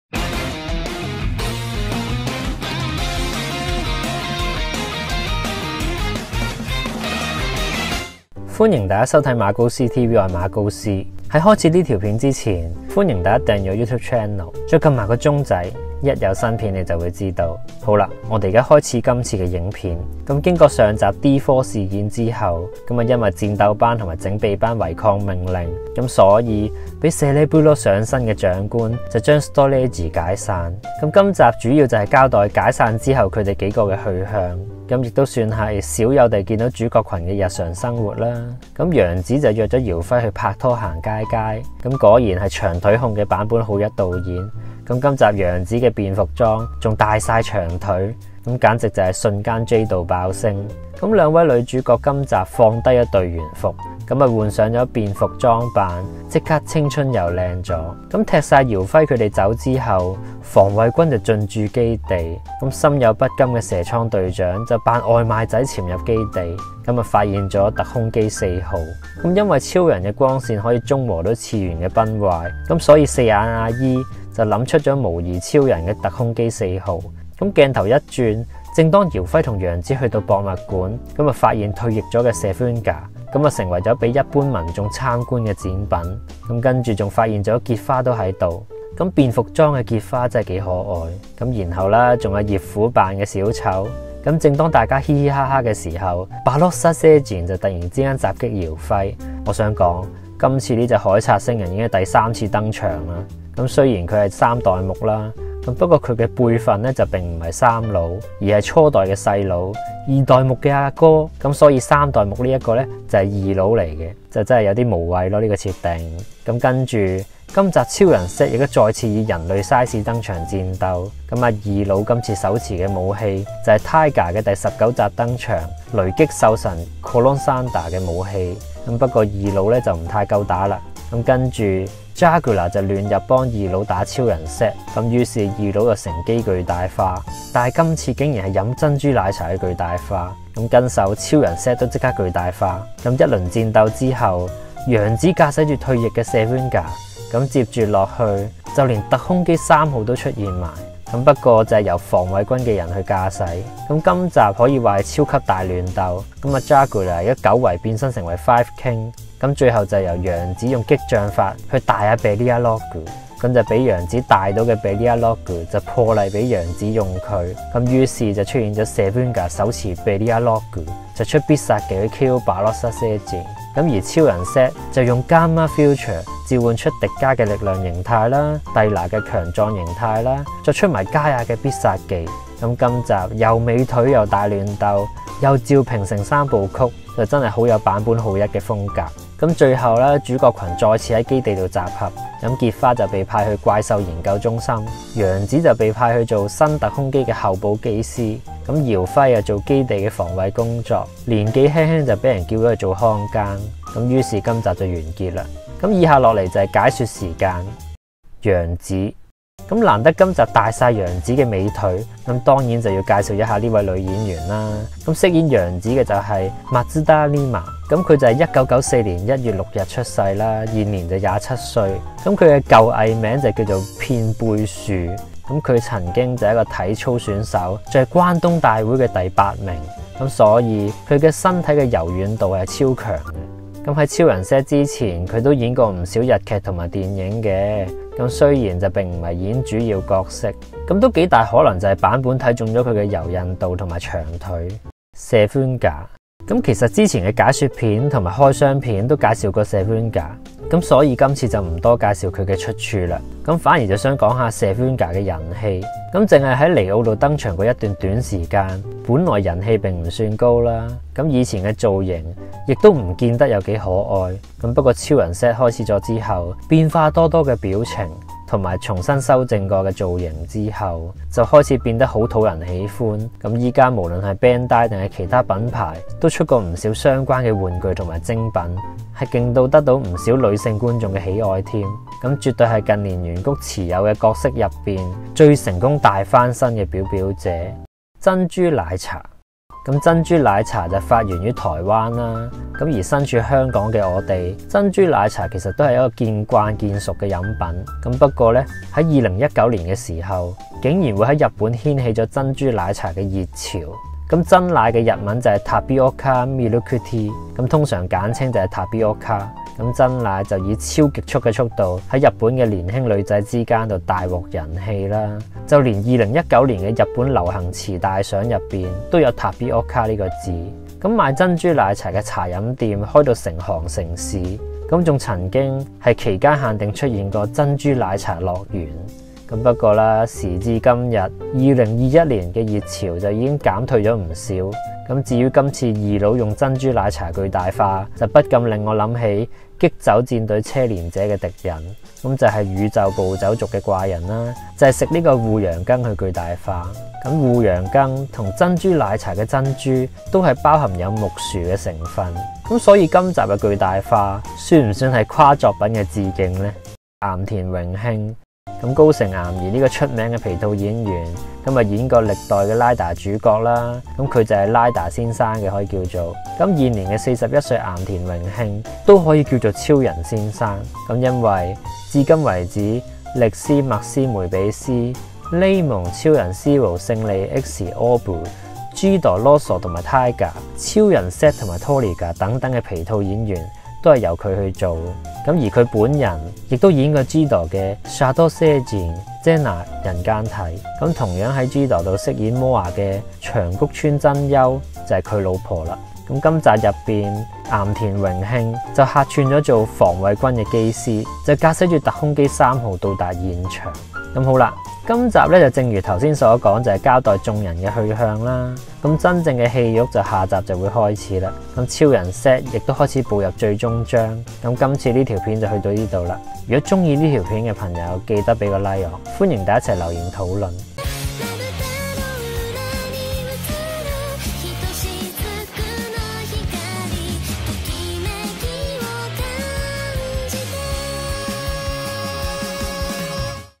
【馬高斯TV】洋子便服晒長腿 / 岩田榮慶本尊演出 / 珍珠奶茶熱潮《 超人Z 》第22話劇情分析 英雄幫 超人力霸王傑特 ultraman ViuTV
話咁快又一個星期過去，又嚟到《超人Z》的睇後感時間，今次第22集就等我同大家過做解說同分析！

歡迎大家留言講低你的感想
或者一齊討論特攝片！

KOICHI MORITA 魔王魂チャンネル@YouTube